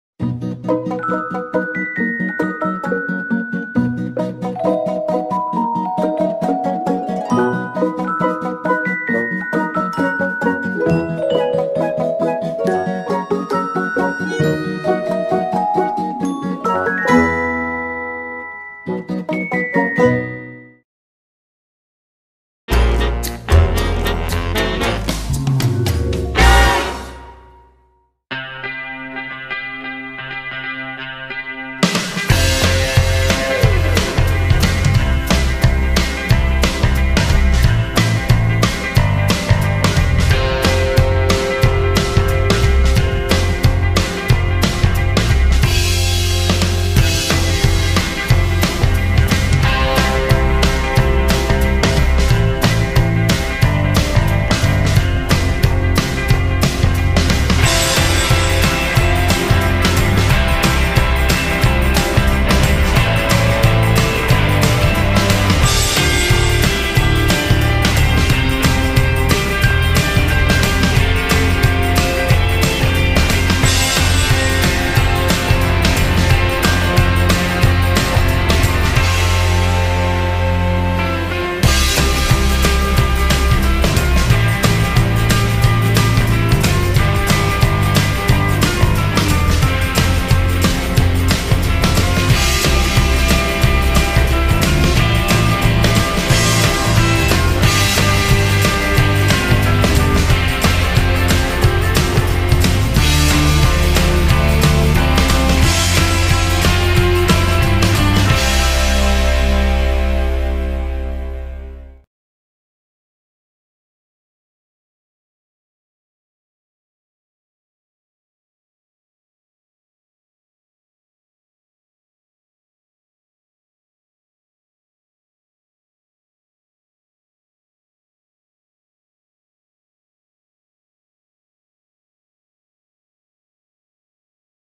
"Le galline con gli occhiali" - Giovanna Pettazzoni Lazzara
L’Opera raccoglie delle brevi storie divertenti che portano con sé un insegnamento importante per i bambini. Dalla gallina Coccodè, che impara cosa implica la menzogna, ai fratellini Leone e Lorenzo Antonino, immersi in mille avventure quotidiane, i personaggi di queste storie parlano ai più piccoli con il loro stesso linguaggio.